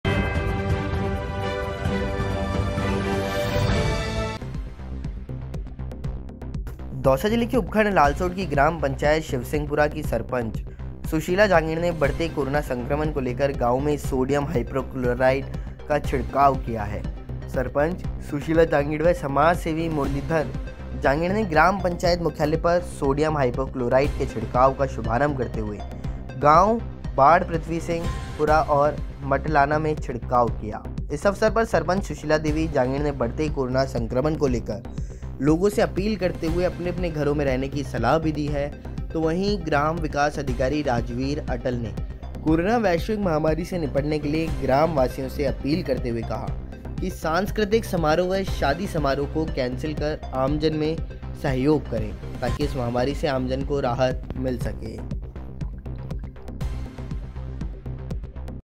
सोडियम हाइपोक्लोराइड का किया छिड़काव Spraying of sodium hypochloride
सरपंच शिवसिंहपुरा व समाजसेवी ने गांव में किया सोडियम हाइपोक्लोराइड का छिड़काव

दौसा जिले के उपखंड लालसोट की ग्राम पंचायत शिवसिंहपुरा की सरपंच सुशीला जांगिड़ ने बढ़ते कोरोना संक्रमण को लेकर गांव में सोडियम हाइपोक्लोराइड का छिड़काव किया !
सरपंच सुशीला जांगिड़ व  समाजसेवी मुरलीधर जांगिड़ ने ग्राम पंचायत मुख्यालय पर सोडियम हाइपोक्लोराइड के छिड़काव का शुभारंभ करते हुए गांव बाढ पृथ्वी सिंह पुरा और मटलाना  में छिड़काव किया !

इस अवसर पर सरपंच सुशीला देवी जांगिड़ ने बढ़ते कोरोना संक्रमण को लेकर लोगों से अपील करते हुए अपने अपने घरों में रहने की सलाह दी !
वहीं ग्राम विकास अधिकारी राजवीर अटल ने कोराना वैश्विक महामारी से निपटने के लिए ग्राम वासियों से अपील करते हुए कहा कि सांस्कृतिक समारोह व शादी समारोह को कैंसिल कर आमजन में सहयोग करें ! ताकि इस महामारी से आमजन को राहत मिल सके !